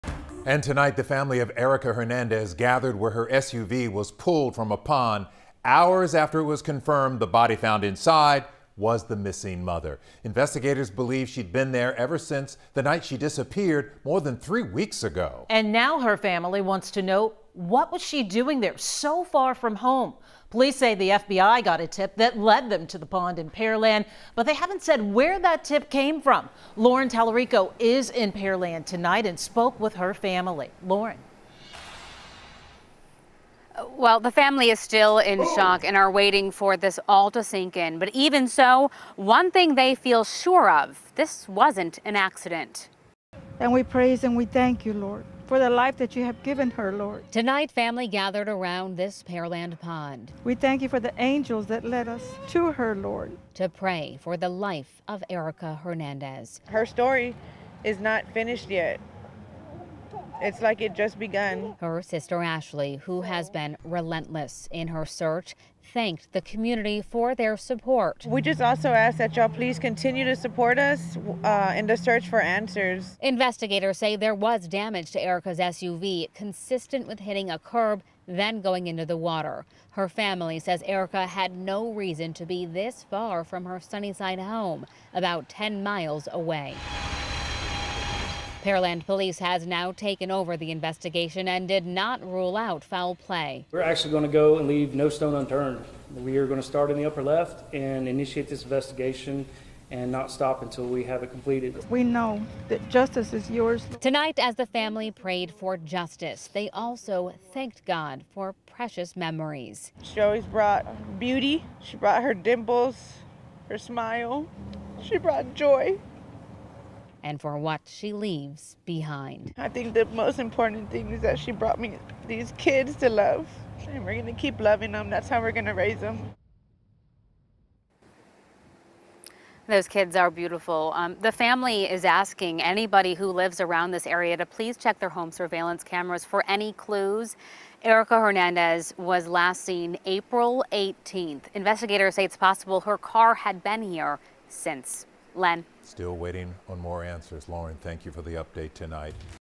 Questions remain even after Erica Hernandez's body was found in her submerged SUV
Police found her SUV yesterday, and today, the remains were identified as those of the missing mother.